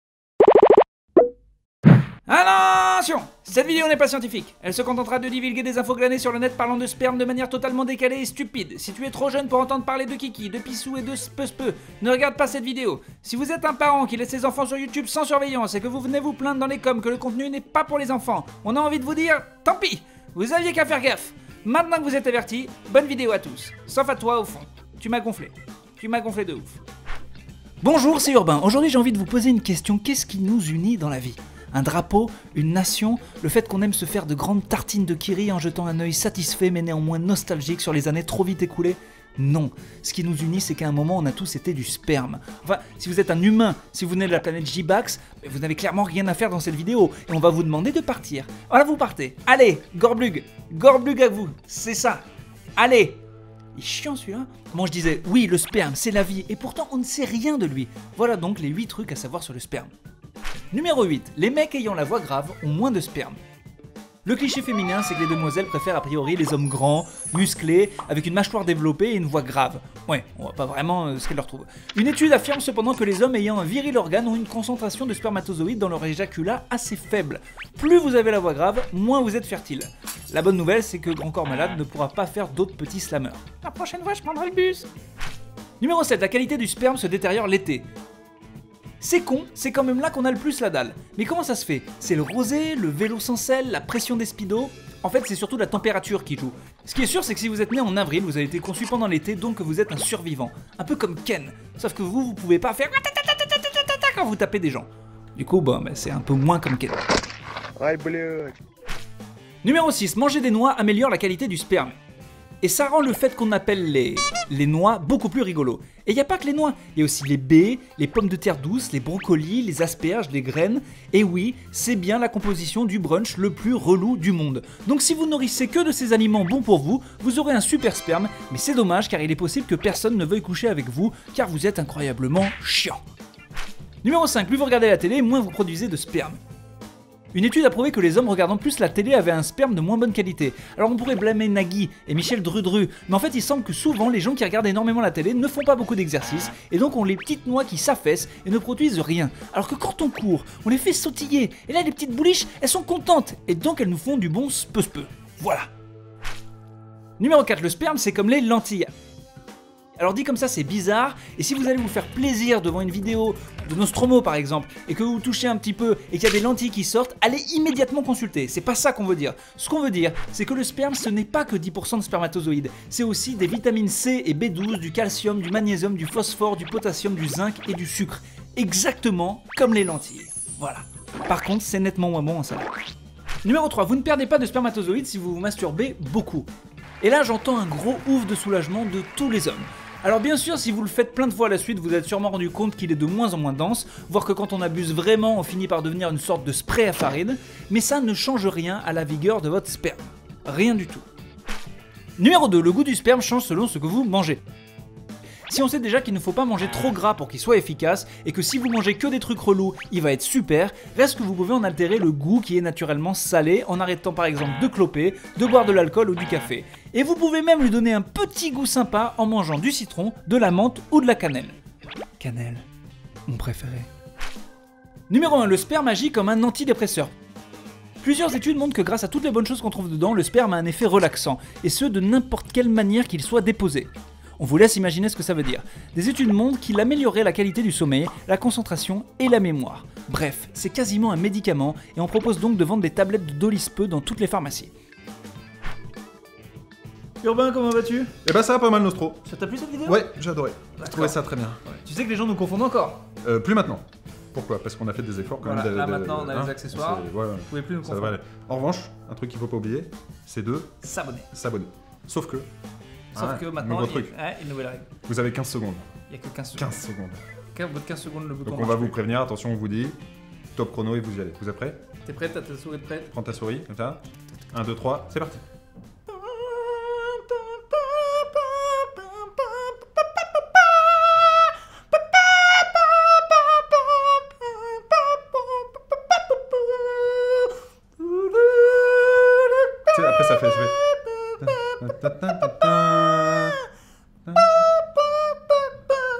Top 8 des trucs à savoir sur le sperme
On ne sait pas assez de choses sur le sperme alors qu'il y a des tonnes de trucs utiles à savoir sur lui ! Sa fonction, son utilité, sa couleur préférée, quel Star Wars il préfère ...
- Si tu as lu le descriptif jusqu'au bout, dis ornithorynque en commentaire !